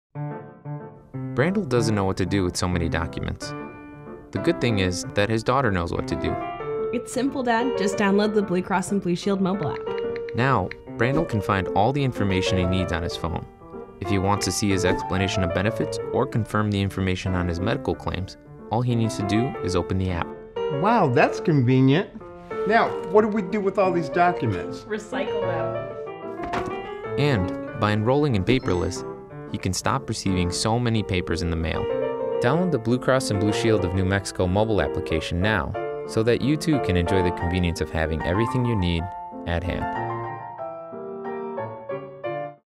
Go digital with the Blue Cross and Blue Shield of New Mexico mobile app!
Wish you didn’t get so much paper in the mail? Go digital and download the BCBSNM Mobile App now. Get all the access and information you need right at your fingertips!